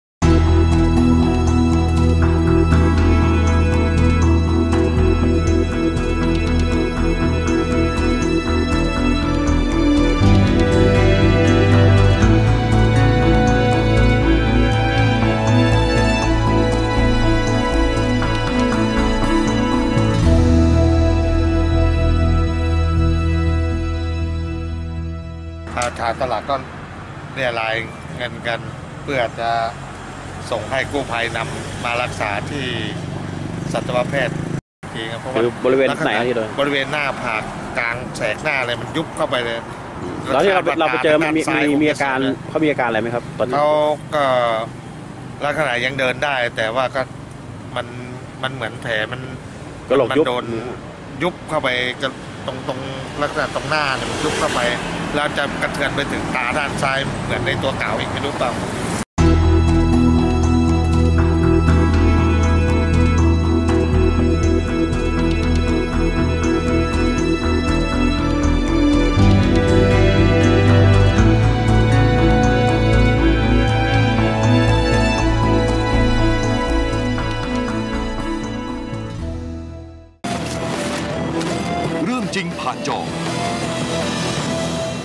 เรื่องจริงผ่านเฟส เรี่ยไรช่วยสุนัข
เรื่องจริงผ่านเฟส ชาวบ้านตลาดสดใน อ.สวรรคโลก จ.สุโขทัย ช่วยกันเรี่ยไรเงินนำส่งให้มูลนิธิเพื่อช่วยรักษาสุนัขที่โดนฟันเป็นแผลกลางหน้าผากลึกจนเห็นกะโหลก หายไวๆนะจ๊ะ หมาน้อย